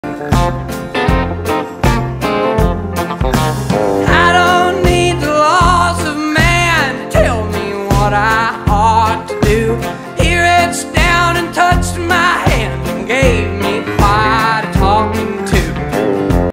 The Laws of Hooman 🤬🐕🐄
Off leash and gone. Took off across 100 acres! AirTag lost. One dirty doggo! 🐾🐾🐾🐾